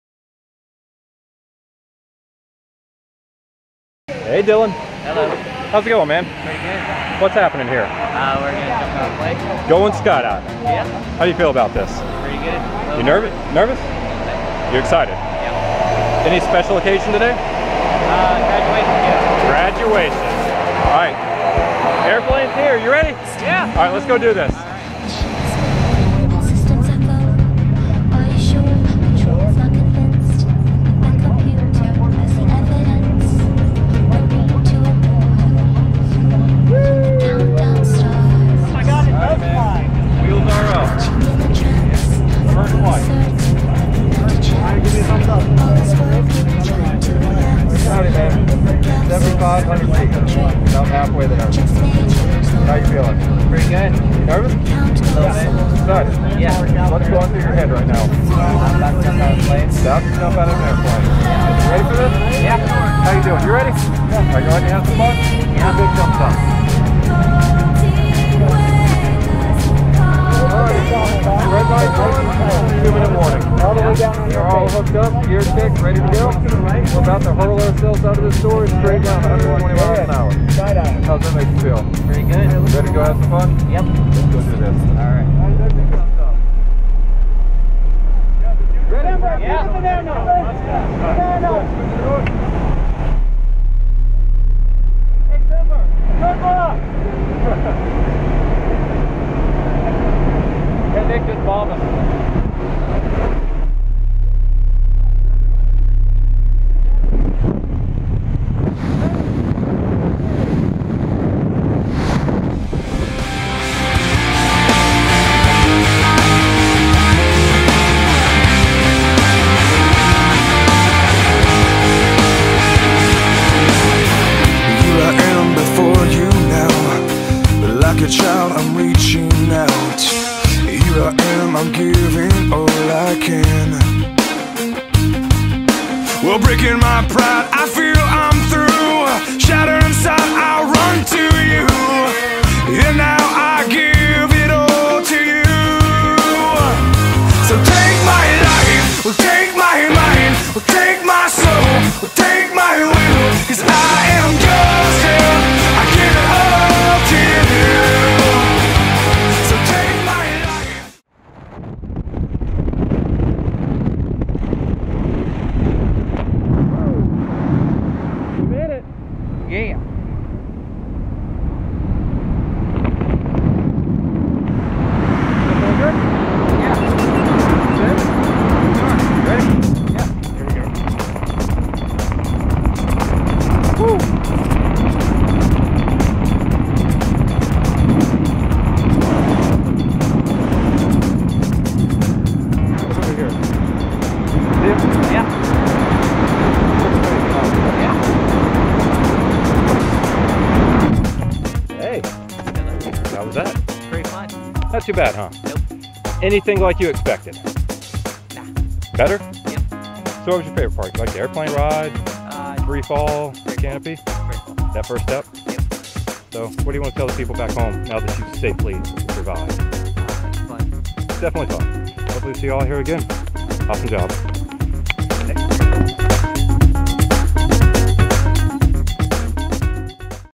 Dylan Farris - Jumps at Skydive Panama City
Dylan Farris - makes a jump at Skydive Panama City